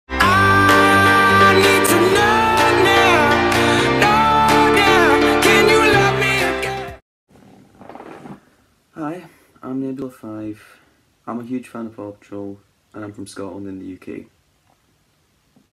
Meet N3BULA V (Reviewing Fan Made Paw Patrol Videos, Coming Soon)
Hello, for those unaware I’m N3BULA V, I’m a huge fan of paw patrol, big fan of Silver the hedgehog and I live in Scotland (UK). This introduction is for an upcoming series I’ll be participating in alongside other huge fans, Da3picMeat and Andrew Gamer GL. This is the first judge for this new series, reviewing fan made paw patrol videos is coming soon, stand by.